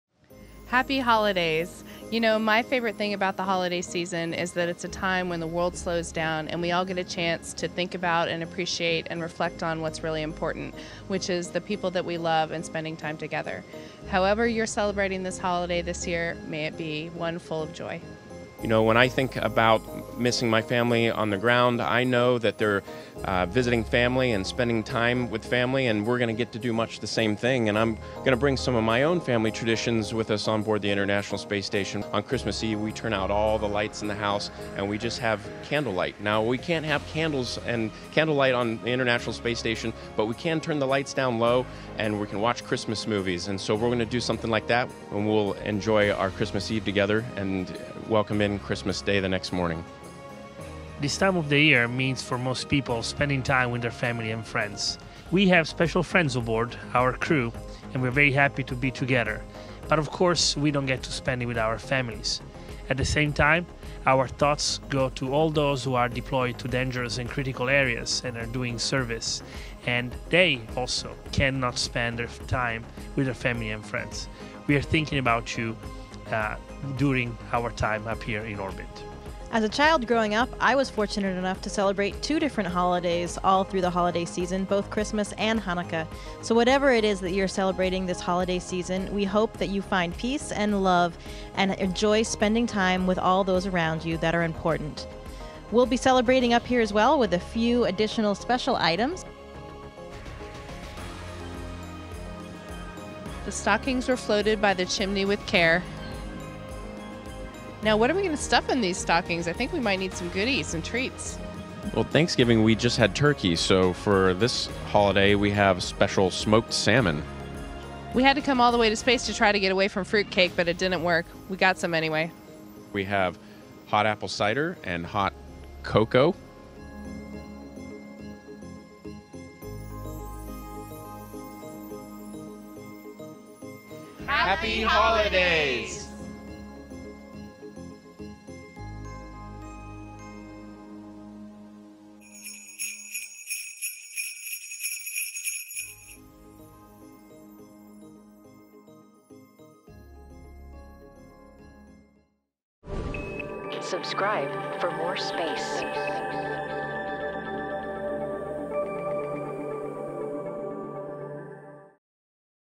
Expedition 61 Christmas Message
Although the astronauts aboard the International Space Station are spending the holidays apart from their families, they have the holiday spirit — and more than just a few of the most important festive supplies. Commander Luca Parmitano and NASA astronauts Christina Koch, Drew Morgan and Jessica Meir share their message for the holidays wrapping up 2019.